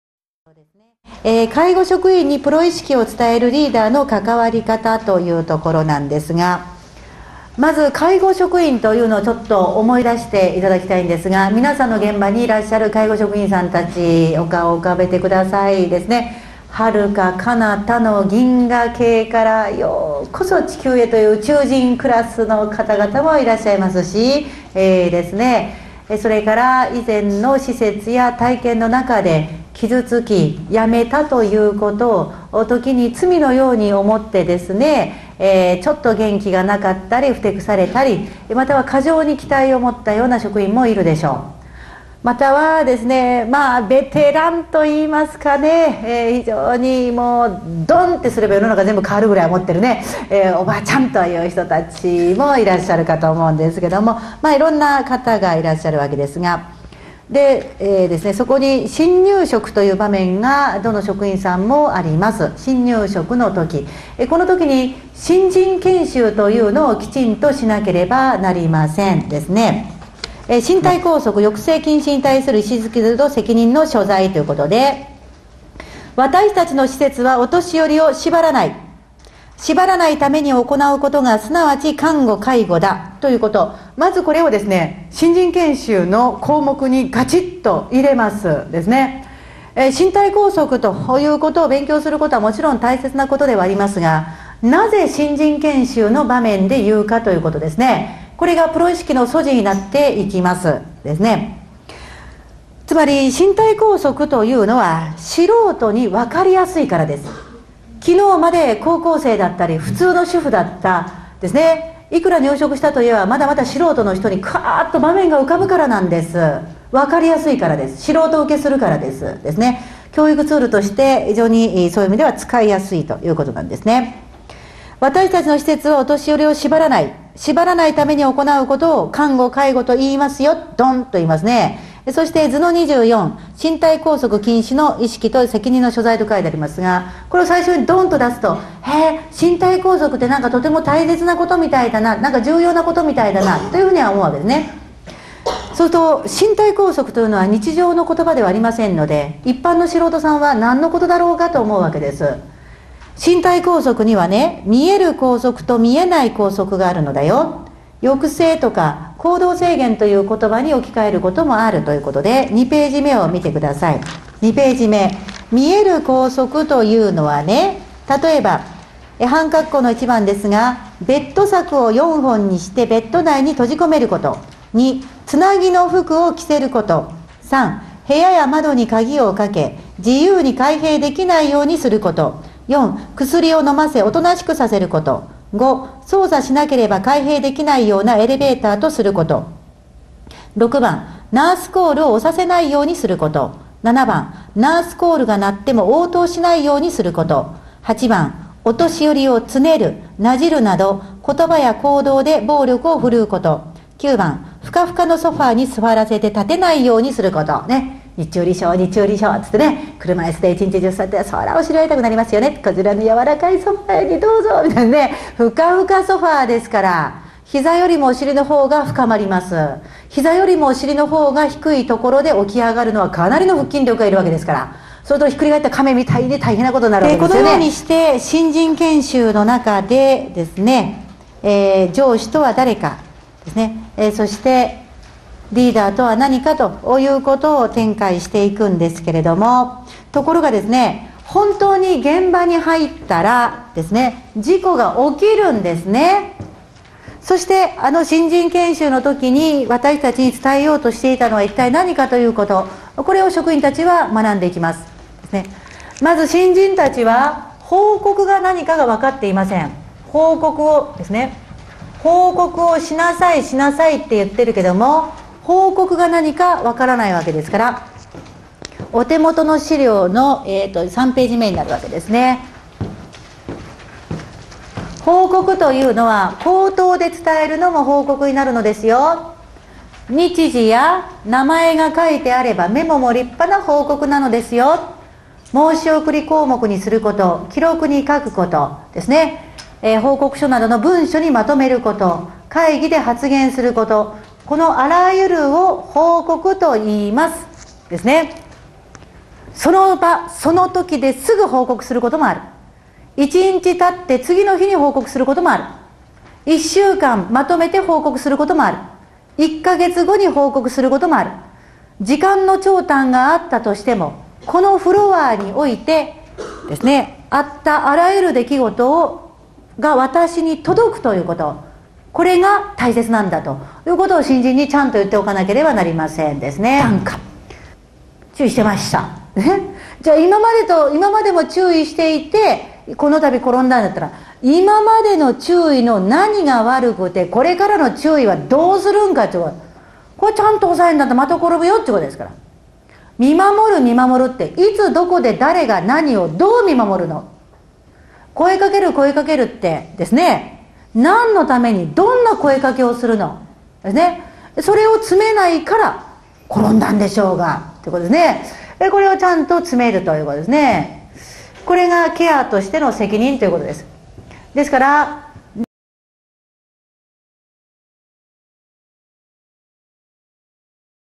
介護職員にプロ意識を伝えるリーダーのかかわり方
一般常識の通用しない宇宙人、他の職場からの転職者など、さまざまな経験と知識と個性を持ったスタッフにプロ意識を持ってもらうためのリーダーのあり方を解説しています。
e-講演.tvで販売しているＤＶＤのサンプル動画です。